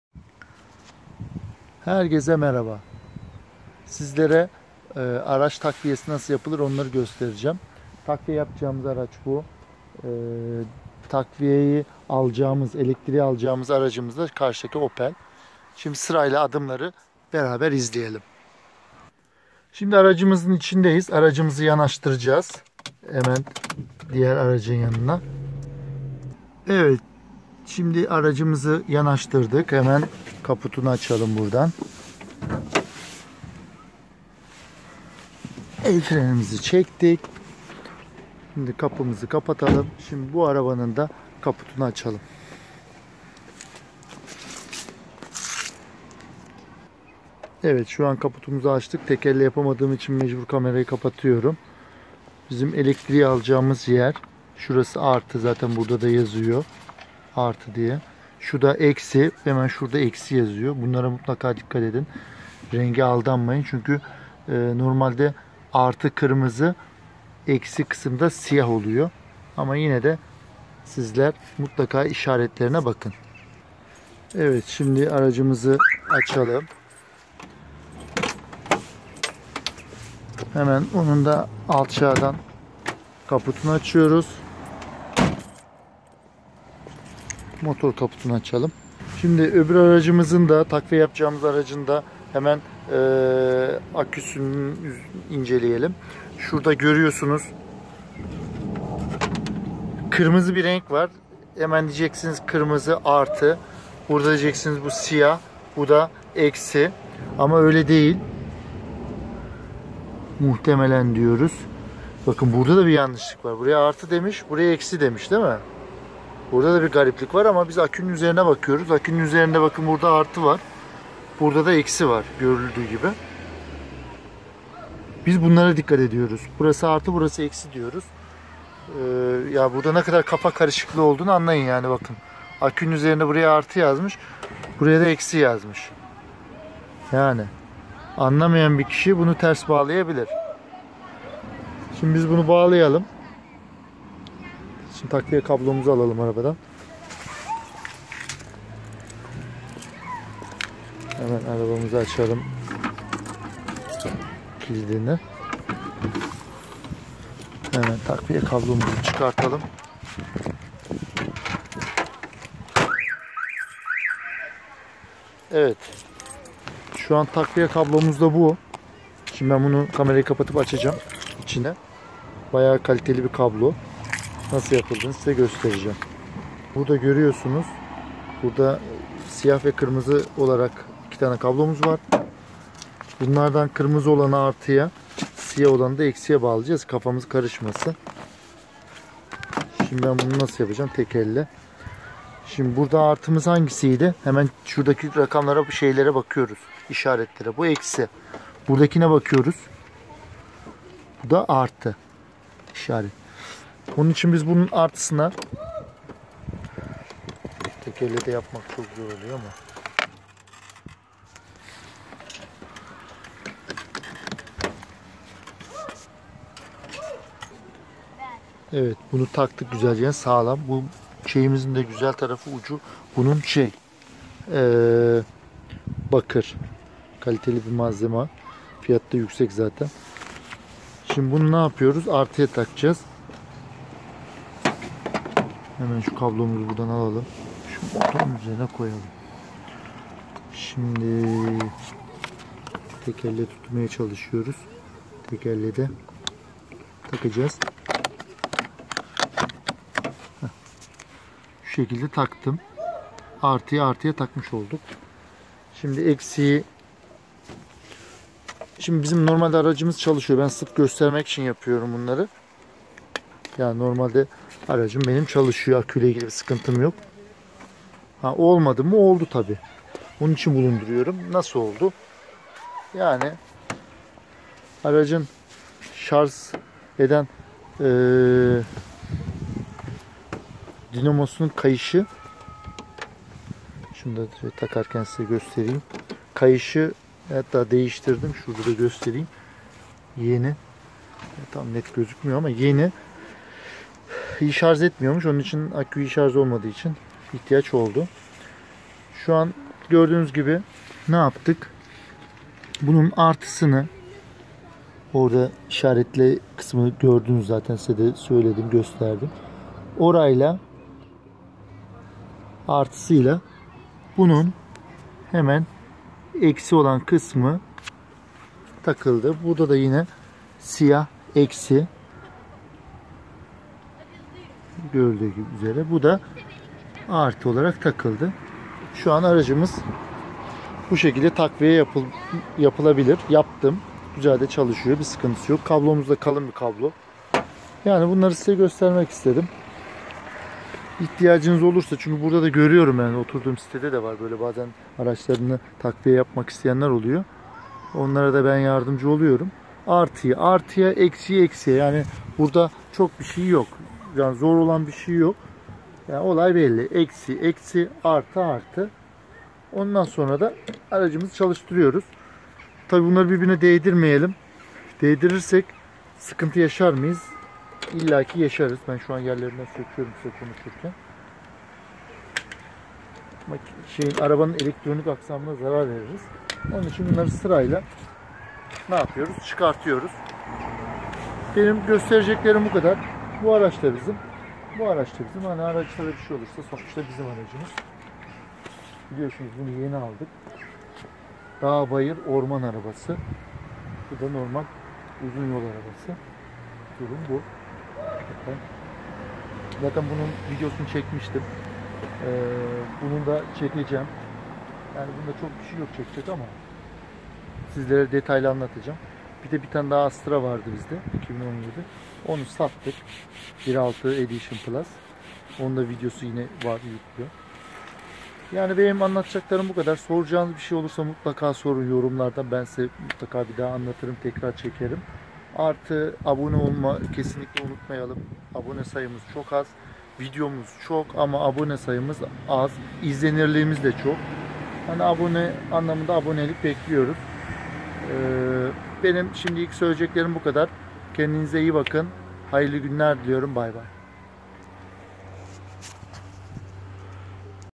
Araçtan Araca Akü Takviyesi Nasıl Olmalıdır? Araba Aküsü nasıl takviye yapılır
Araç AKÜ takviyesi nasıl yapılır Araç yolda kaldığında akü takviyesi
Araçtan Araca Akü Takviyesi Nasıl Olmalıdır?